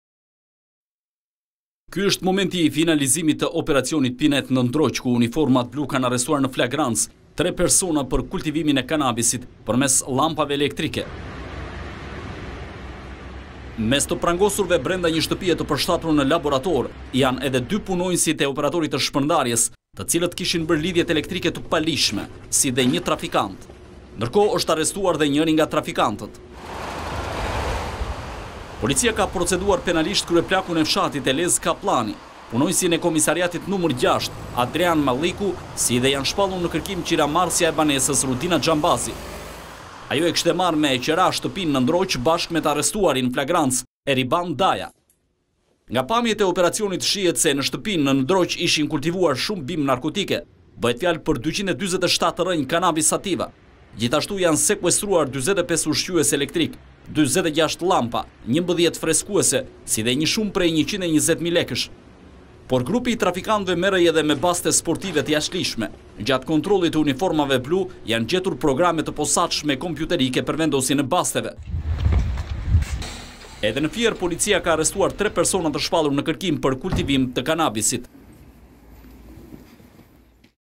Zbulohet shtëpia e "barit" në Tiranë; ne pranga dy punonjës te OSHEE, "zhduket" një grua -
Uniformat blu kanë arrestuar në flagrancë 3 persona për kultivimin e kanabisit përmes llambave elektrike gjate operacionit te koduar "Pinet" ne Ndroq.Mes personave të kapur në flagrance janë edhe dy punonjës të OSHEE-së, të cilët kishin bere lidhje elektrike të paligjshme dhe një rafikant.Ndërkohë janë proceduar penalisht, Kryeplaku i fshatit, Elez Kapllani, punonjësi i komisariatit nr.6, Adrian Malliku, si dhe janë shpallur në kërkim qiramarrësja e banesës, Rudina Xhambazi.Ajo e kishte marrë me qira banesën bashkë me të arrestuarin në flagrancë, Eriban Daja.Nga pamjet e operacionit shihet se në shtëpinë në Ndroq ishin kultivuar shumë bimë narkotike. Bëhet fjalë për 247 rrënjë kanabis sativa.Gjithashtu janë sekuestruar 45 ushqyes elektrikë, 46 llamba elektrike, 11 freskuese si dhe një shumë prej 120.000 lekë.Por grupi I trafikanteve merrej edhe me baste sportive te jashtëligjshme. Gjatë kontrollit të uniformave blu janë gjetur edhe programe për vendosjen e basteve.Ndërkohë mëngjesin e kësaj të shtune, policia e Fierit ka arrestuar edhe 3 persona të shpallur në kërkim, për kultivimin e kanabisi.